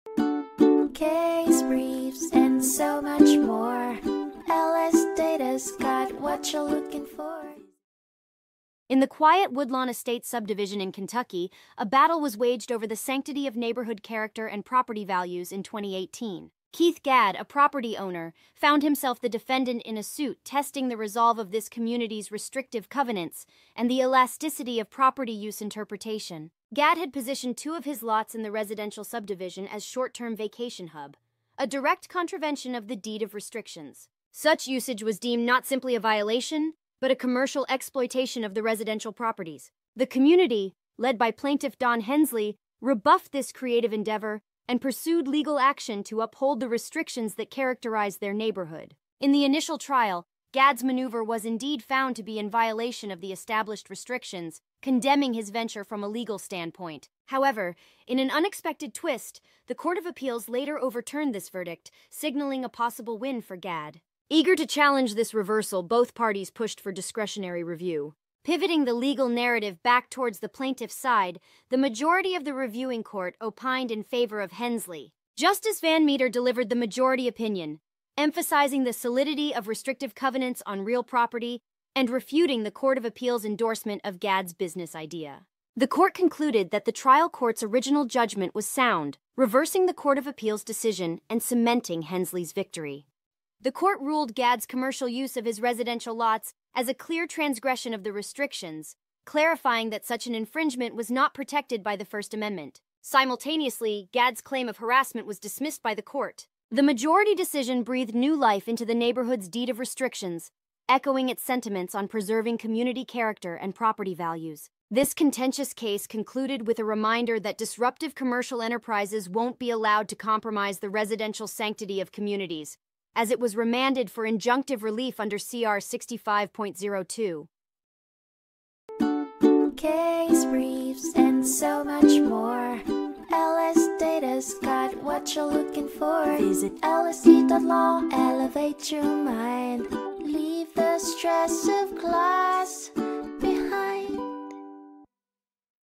Hensley v. Keith A. Gadd & JHT Props., LLC (2018) Overview | LSData Case Brief Video Summary
In this case, there was a Deed of Restrictions for a residential subdivision that prohibited the use of properties for short-term vacation rentals and commercial purposes. The defendant violated this restriction by advertising two lots for short-term residential use. The trial court found the defendant guilty, but the Court of Appeals reversed it. Both parties requested discretionary review.

Hensley v. Keith A. Gadd & JHT Props., LLC (2018)
St. Louis Court of Appeals
560 S.W.3d 516